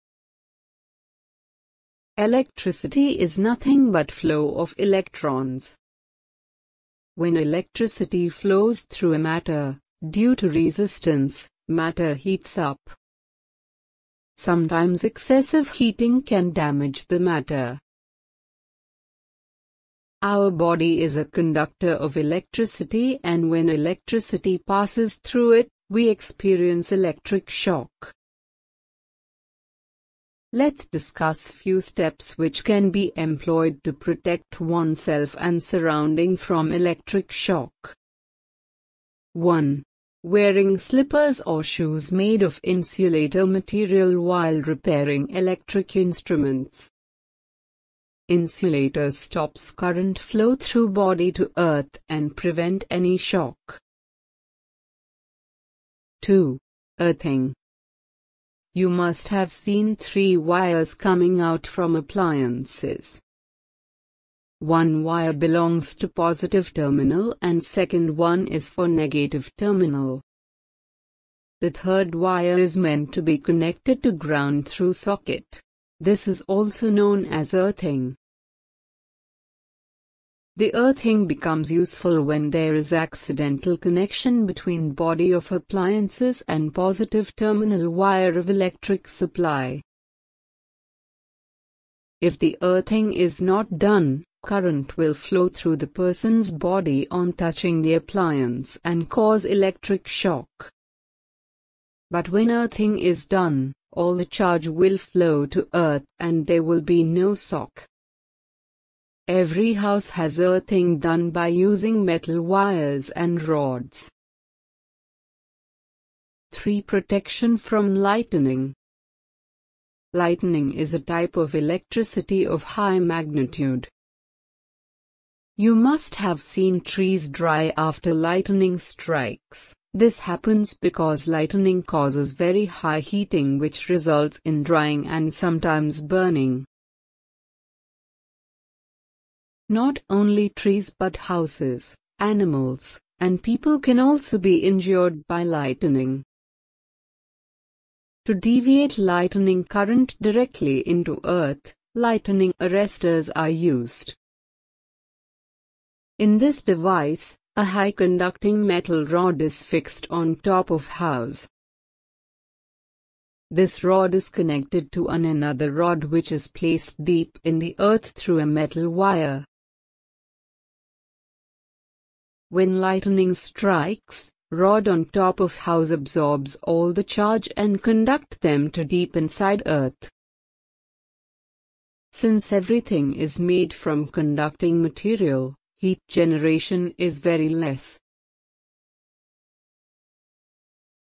Understanding Electrical Safety: Protecting Against Electric Shock and Lightning
Join us as we explore the fascinating world of electricity and learn how to safeguard ourselves and our surroundings from electric shock and lightning. Discover how electricity, which is the flow of electrons, can generate heat and potentially damage materials due to resistance. Gain insights into the importance of insulator materials, such as wearing slippers or shoes, to prevent current flow through the body during electrical repairs. Explore the crucial concept of earthing, where the third wire of electrical appliances is connected to the ground, ensuring a safe path for excess current and eliminating the risk of electric shock. Uncover the dangers of lightning and its destructive power, causing dryness, burning, and harm to living beings. Learn about lightning arresters, which channel the immense energy of lightning directly into the ground, minimizing heat generation and protecting structures, animals, and people. Empower yourself with essential knowledge and precautions for a safer electrical environment.